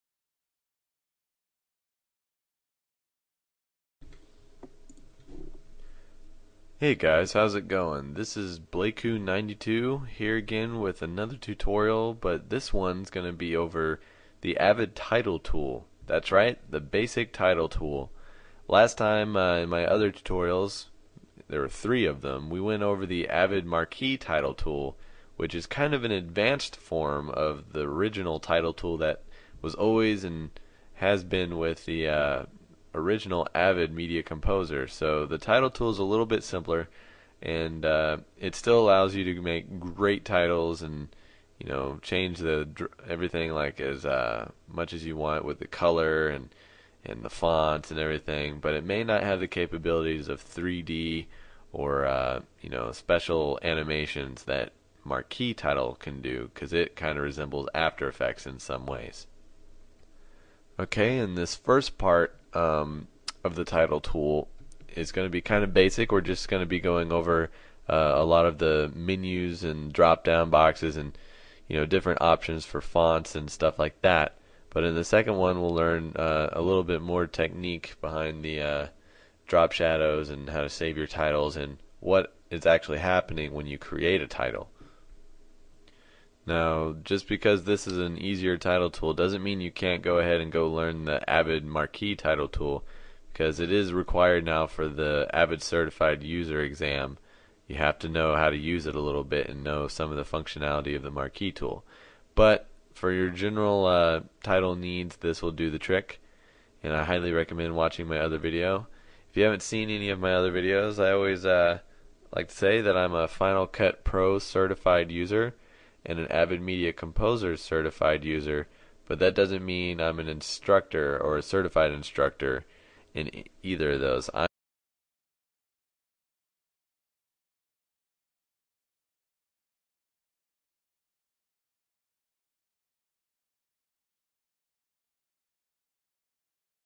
Avid Title Tool Part 1
Part 1 of 2, this class is an overview of the original Avid Title Tool. Built into the interface, Title tool is simple yet powerful.